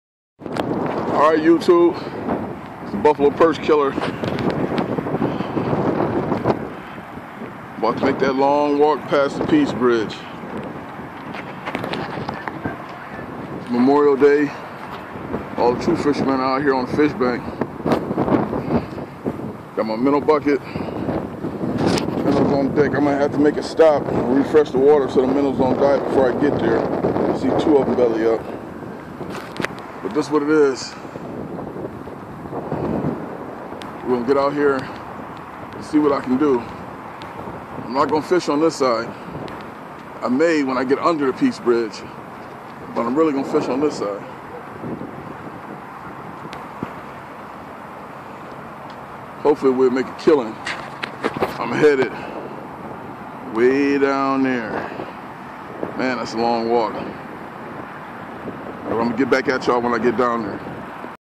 Buffalo perch killers Memorial day fishing trip part 1
Making that long walk under the piece bridge in buffalo new York